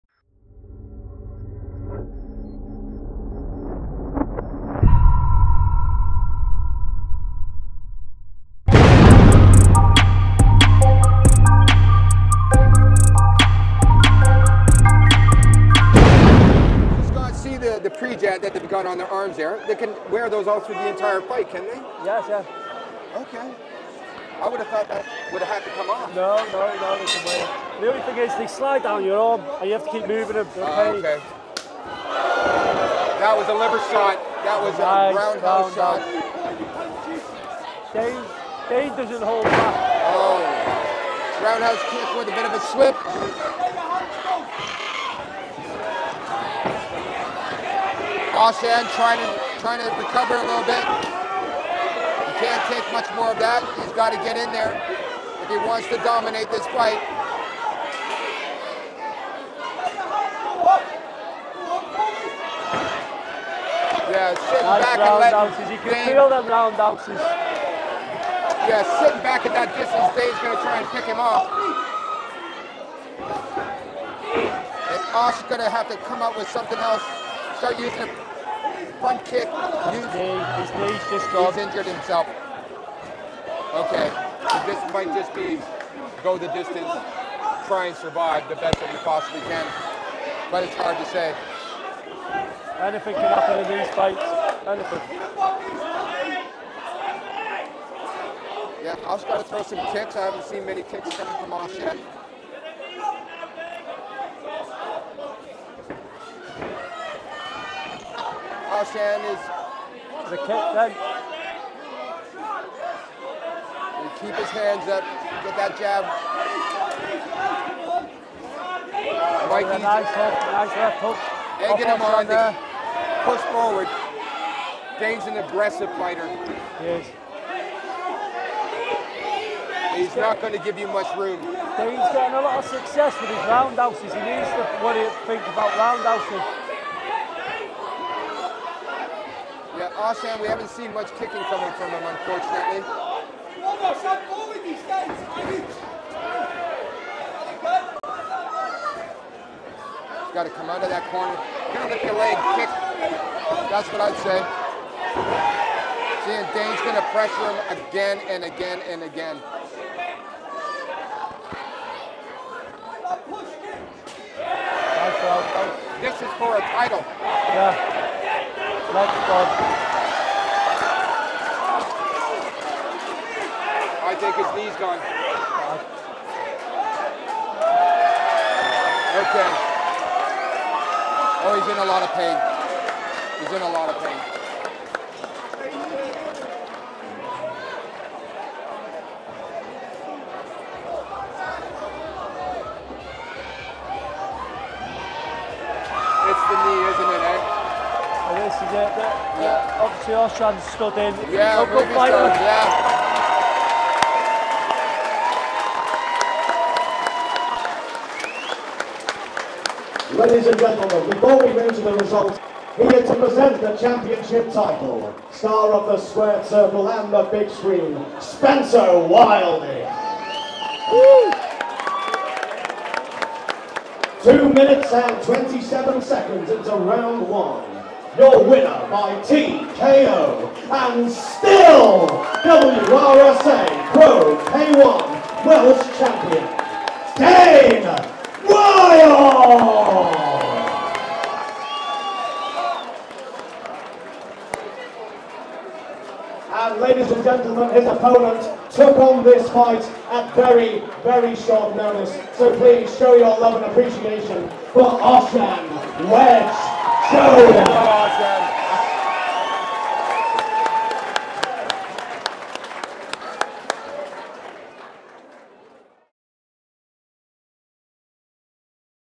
Dayne Wild vs Osian Jones WRSA K1 Welsh Title
The Comeback.

March 25th 2023 in the Broadway Boulevard, Llandudno

Shot and Edited by Robin Abas with help from Lili Abas and Hywel Griffith

MC Ronnie Rowlands
Commentary by Scott Thomas and Gary Elder